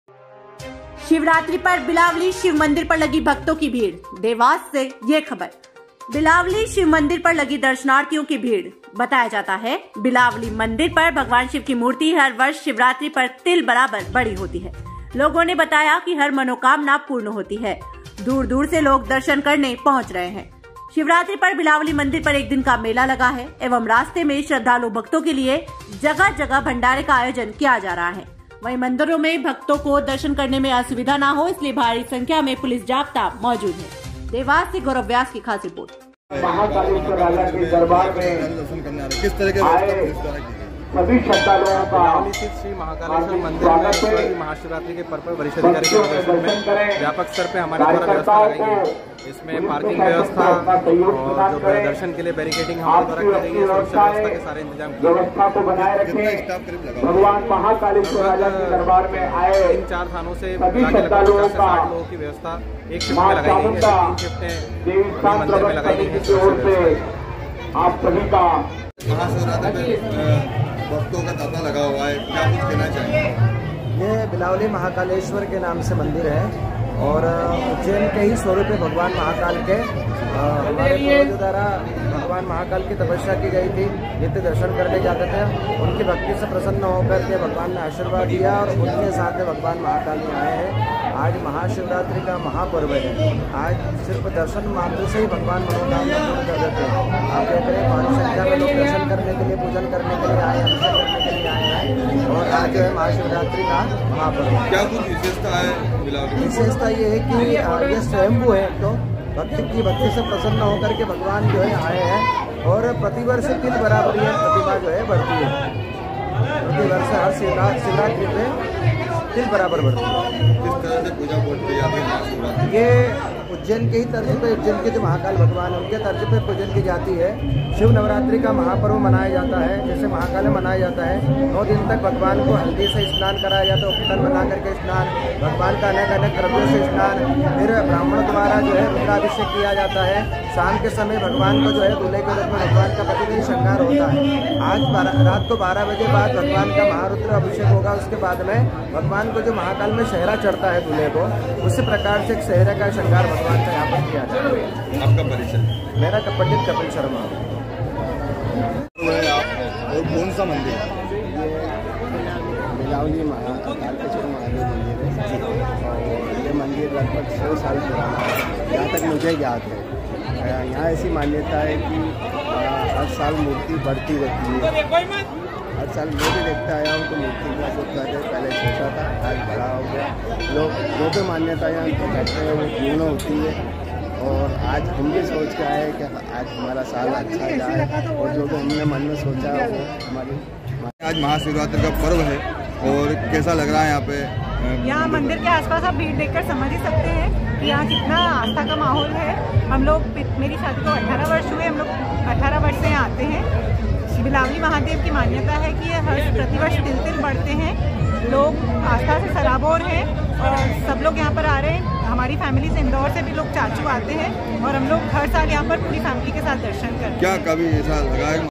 dewasnews - शिवरात्रि पर बिलावली शिव मंदिर पर लगी भक्तों की भीड़ @mpnews_live
शिवरात्रि पर बिलावली  शिव मंदिर पर लगी भक्तों की भीड़

देवास | बिलावली शिव मंदिर पर लगी दर्शनार्थियों की भीड़ बताया जाता है बिलावली मंदिर पर भगवान शिव की मूर्ति हर वर्ष शिवरात्रि पर तिल बराबर बड़ी होती है लोगों ने बताया कि हर मनोकामना पूर्ण होती है दूर- दूर से लोग दर्शन करने पहुच रहे है शिवरात्रि पर बिलावली मंदिर पर एक दिन का मेला लगा  है एवं रास्ते मे श्रद्धालु भक्तों के लिए जगह जगह भंडारे का आयोजन किया जा रहा वही मंदिर में भक्तों को दर्शन  करने  मे कोई असुविधा ना हो इसलिए भारी संख्या मे पुलिस बल भी मौजूद है  |

देवास-  गौरव व्यास